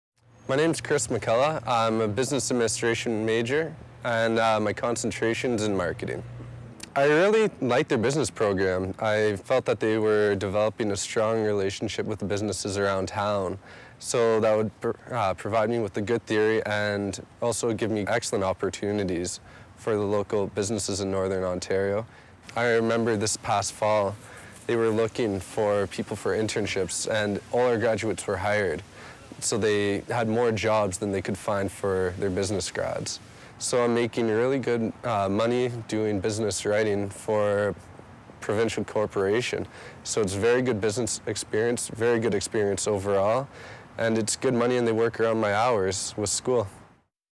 Chris McCullough.mov
Chris McCullough talks about his experience as an Algoma University Business student.